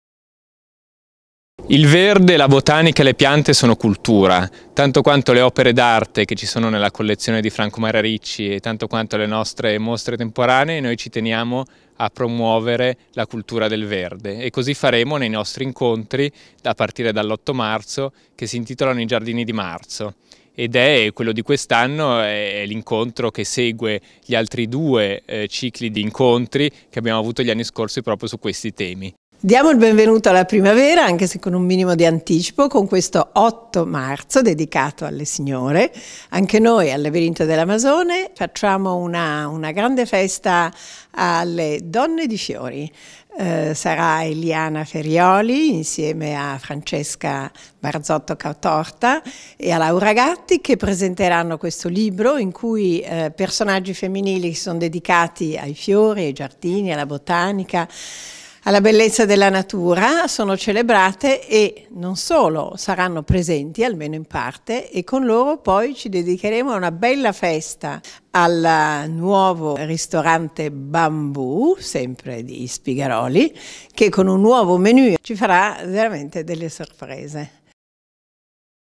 I GIARDINI DI MARZO 2018. Conversazioni sul Verde al Labirinto della Masone
Tre incontri nel Labirinto di Franco Maria Ricci per raccontare e indagare il sorprendente mondo delle piante e dei giardini.

Giovedì 8, 15  e mercoledì 28 marzo alle 18, il Labirinto della Masone con la collaborazione della delegazione FAI di Parma ospita un ciclo di incontri dedicato alla botanica, I giardini di marzo, con interventi di importanti studiosi e connaisseur.